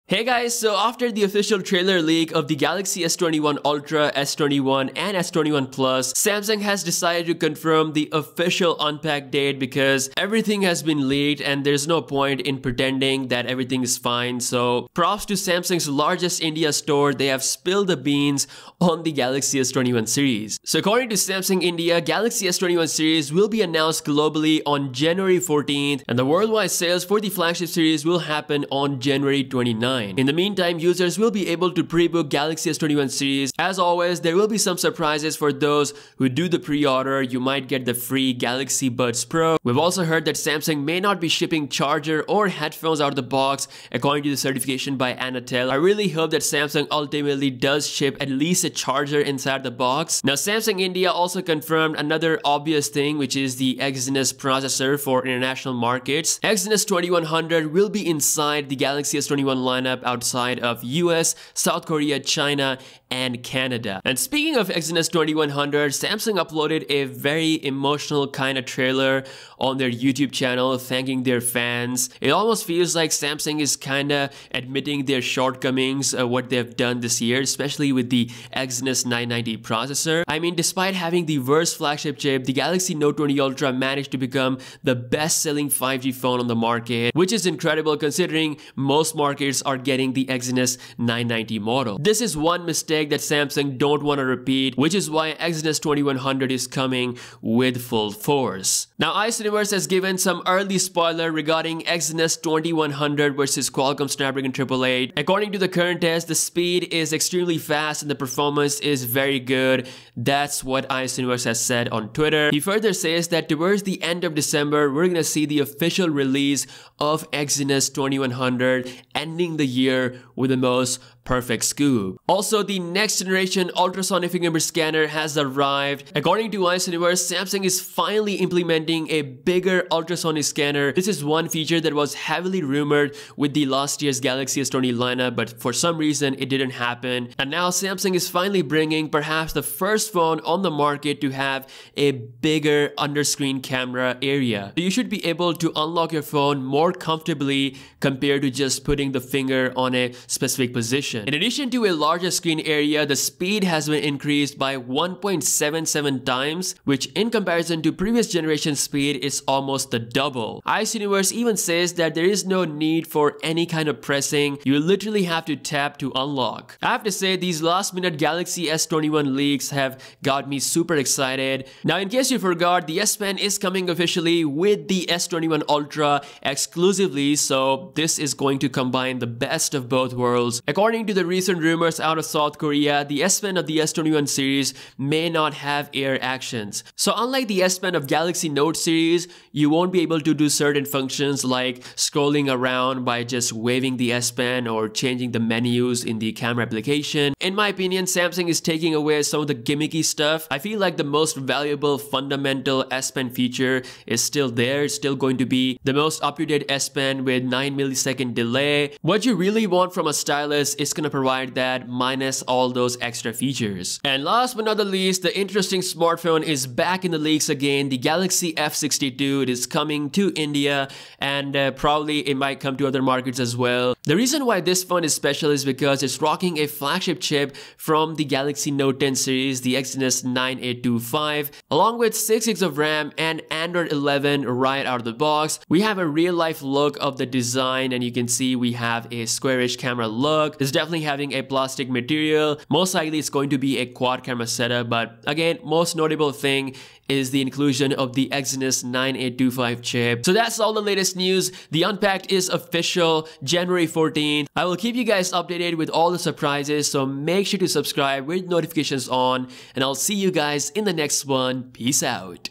Samsung Galaxy S21 - UNPACKED OFFICIAL
Samsung Galaxy S21 Unpacked official January 14th, Exynos 2100 Processor is ready to rumble hard, Galaxy S21 with NEW Fingerprint scanner Galaxy F62 & more.